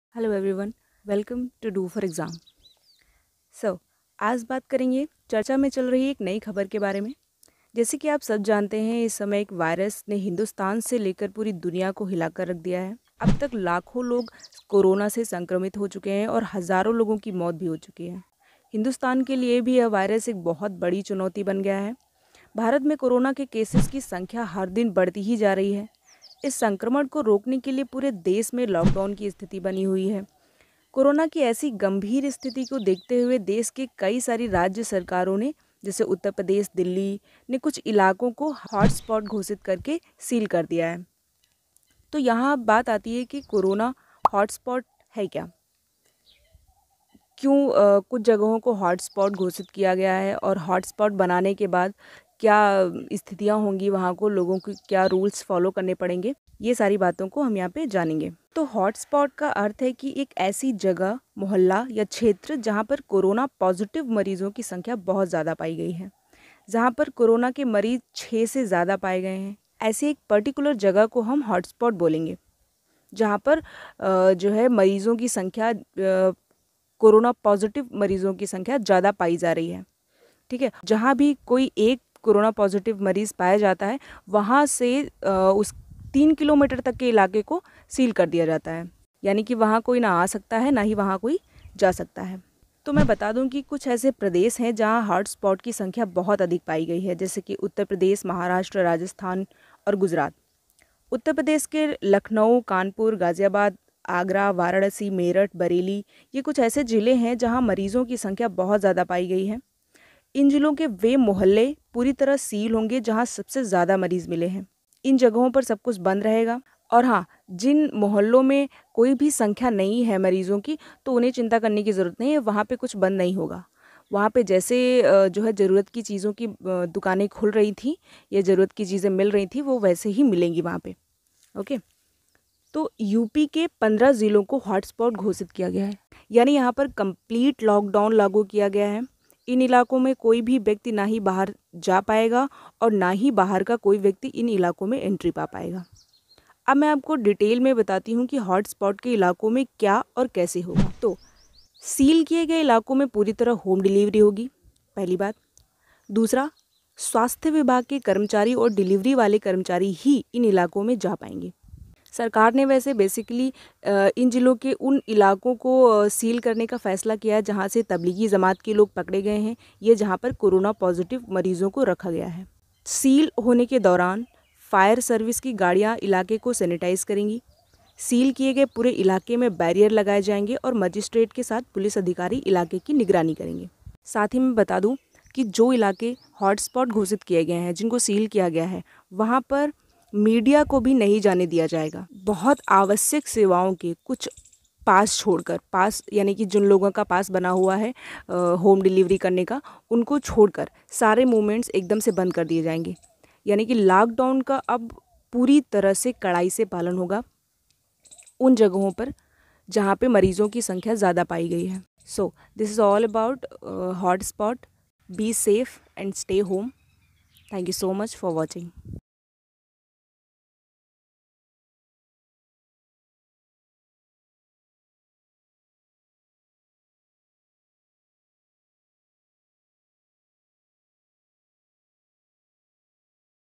क्या होता है Corona Hotspot जो 15 जिलों में पूरी तरह है सील? UP 15 District Hotspot Sealed
क्या है Corona Hotspots? UP 15 District Hotspot Sealed | Yogi on UP lockdown

Kya hai corona hotspot?
Kaun kaun se hotspot bnaye gye hai?
UP me kin kin jagaho ko seal kiya gya?
Lucknow me kin jagaho ko seal kiya gya?

यूपी में कोरोना के बढ़ते संक्रमण को देखते हुए फैसला लिया गया है कि 15 जिलों के हॉटस्पॉट को सील किया जाएगा। प्रमुख सचिव गृह अवनीश अवस्थी ने बताया कि लोगों को हर हाल में बाहर निकलने से रोका जाएगा। यह फैसला पंद्रह अप्रैल तक के लिए लिया गया है।

हॉटस्पॉट का अर्थ यह है कि वह जगह जहां सबसे अधिक कोरोना पॉजिविट मरीज पाए जा रहे हैं। लखनऊ, गौतमबुद्ध नगर, गाजियाबाद, आगरा, कानपुर, वाराणसी, शामली, मेरठ, सीतापुर, बरेली, बुलंदशहर, फिरोजाबाद, बस्ती, सहारनपुर और महाराजगंज ऐसे जिले हैं यहां मरीजों की संख्या ज्यादा है। इन जिलों के वे मोहल्ले पूरी तरह सील होंगे जहां सबसे  ज्यादा मरीज मिल रहे हैं।